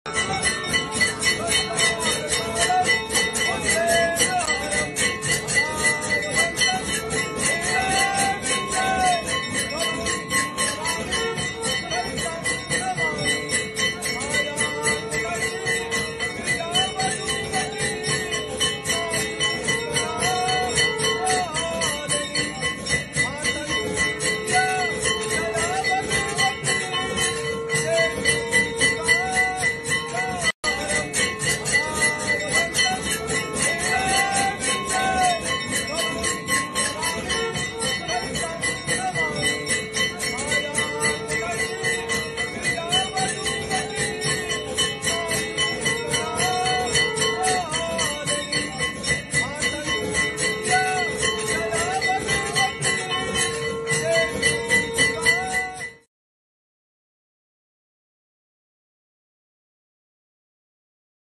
Friday, 8 April 2022 | Saptmi Evening Darshan | Jai Kalka Maa
🛕🙏🙇 DailyDarshan - Navratri Day 7 🙇🙏🛕
🥀🌸 || Friday, 8 April 2022 || 🌸🥀

Our sites where we upload DailyDarshans of Shri Kalka Maa ❤🙏🌸 Join us & Let's spread the name of Shri Kalka Maa 🥰